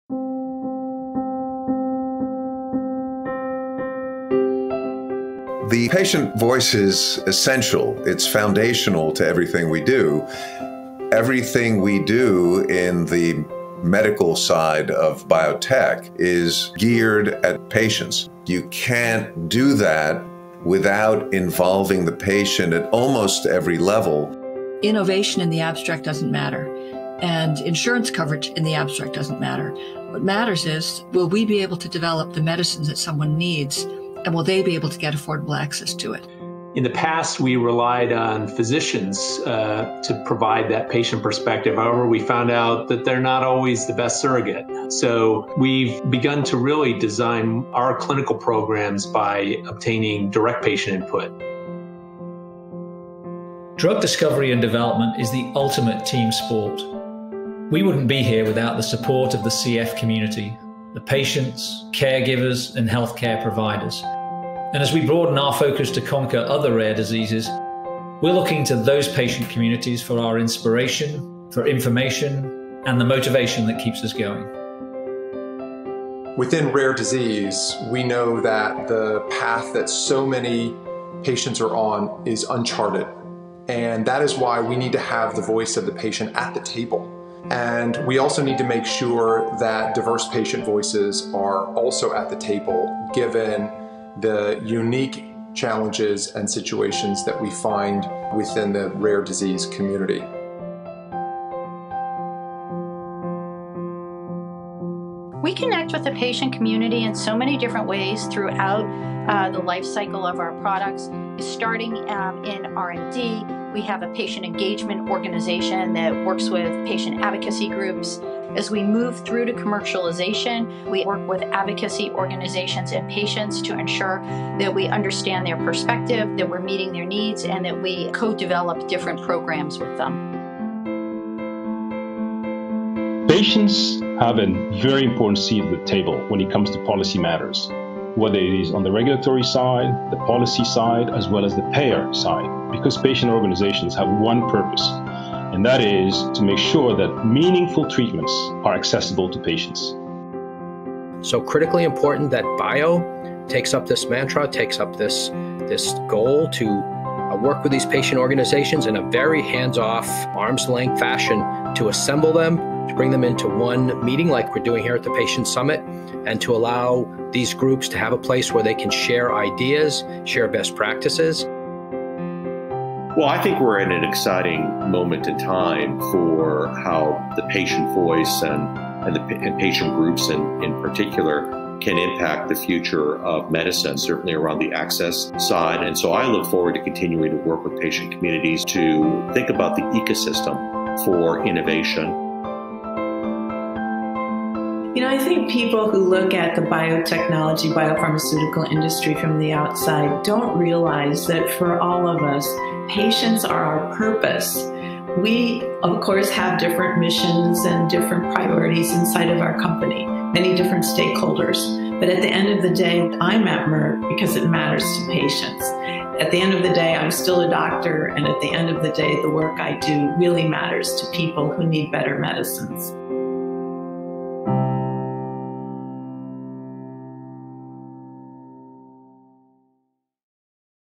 Biotech Leaders Stress the Importance of the Patient Voice
Ten biotech leaders weigh in on why the patient voice is important throughout the life cycle of drug development and drive home how patient advocacy can help shape government policy.